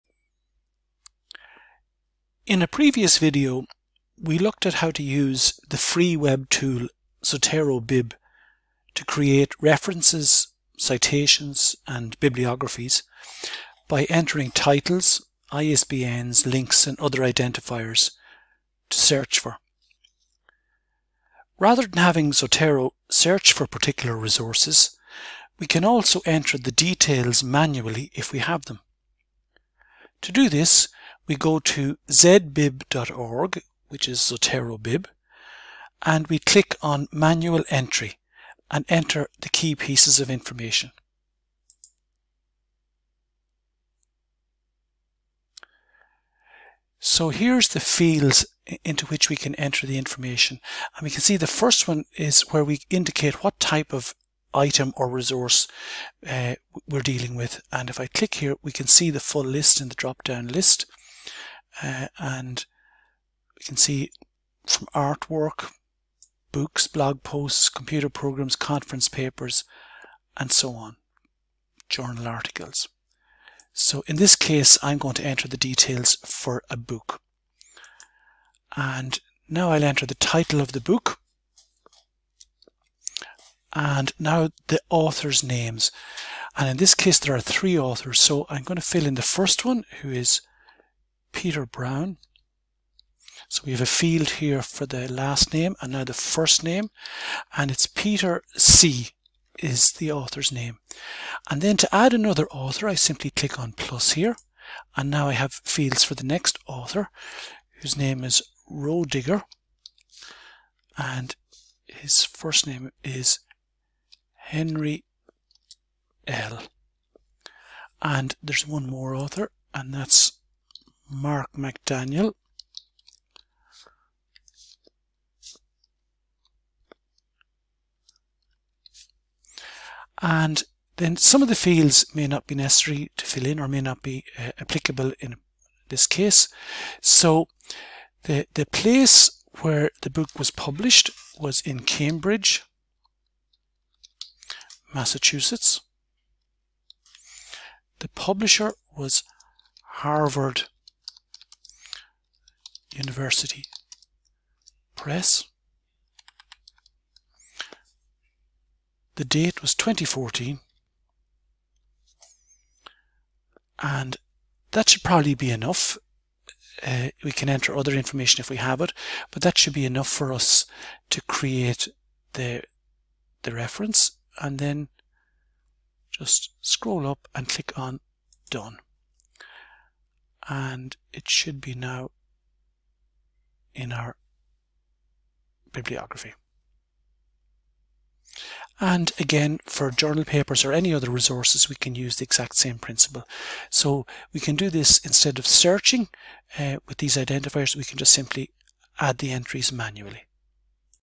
ZoteroBib - Entering details manually
In this video we look at how to enter resource details manually in ZoteroBib.